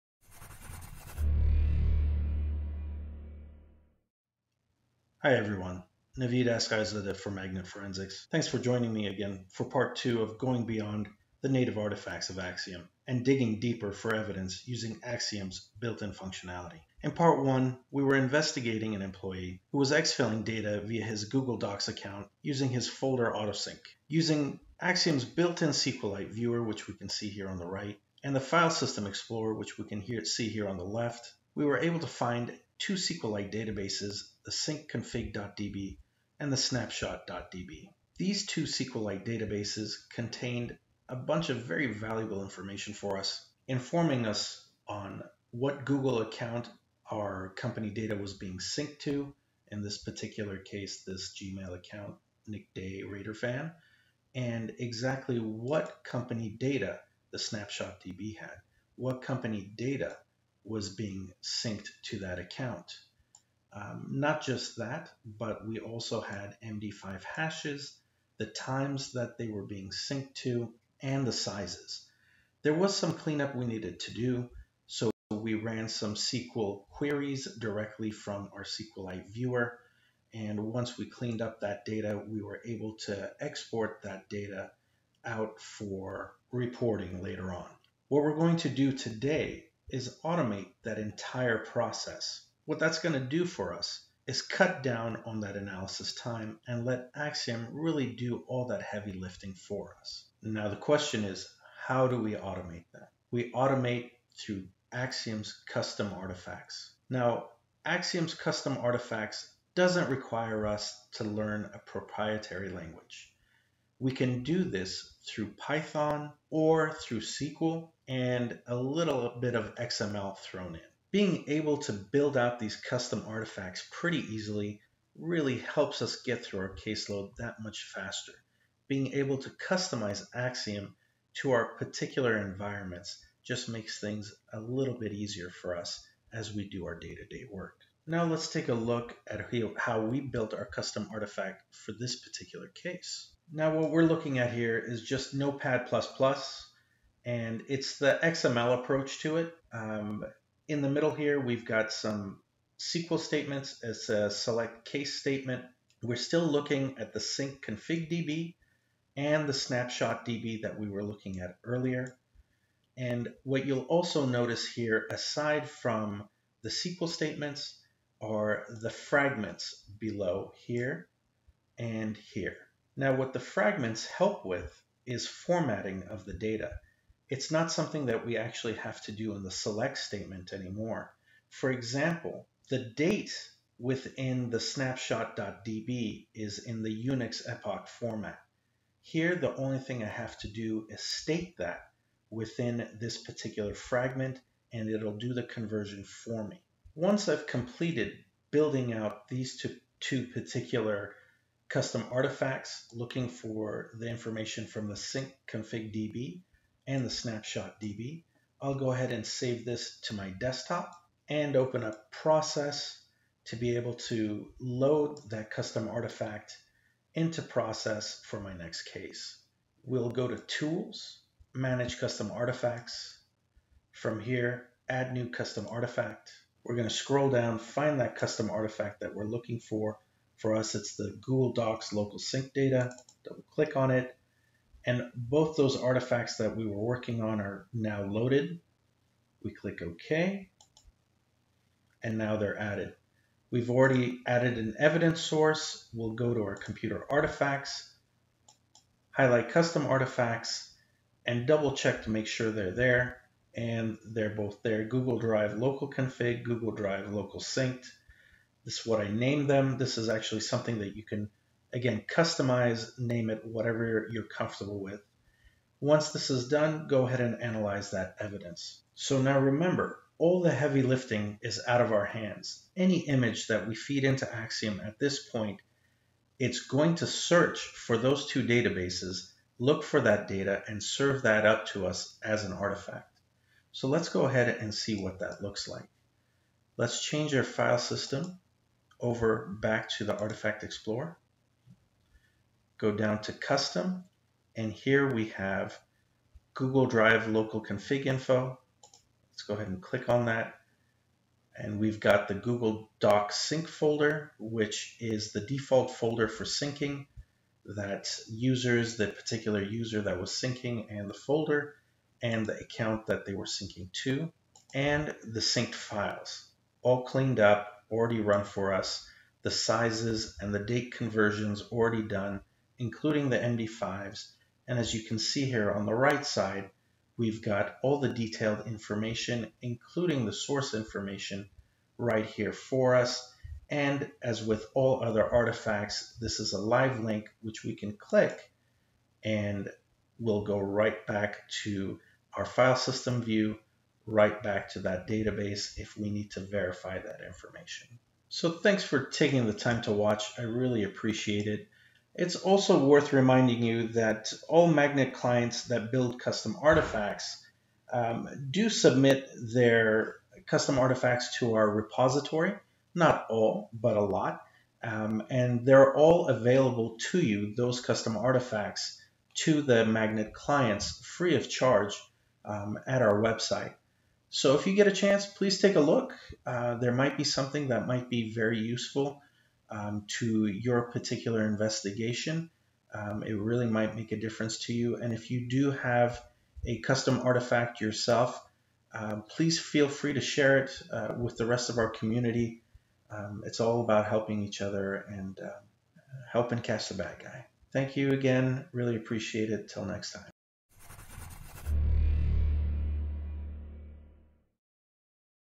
AXIOM at Work: Digging Deeper with Custom Artifacts (Pt. 2)
AXIOM at Work is a video series highlighting specific instances where Magnet AXIOM can be beneficial in your corporate investigations. In the second part of this two-part installment, Navid Asgharzadeh, Solutions Consultant, talks about custom artifacts in Magnet AXIOM, and using them to automate within your case.

To learn more about how you can use AXIOM in your corporate investigations,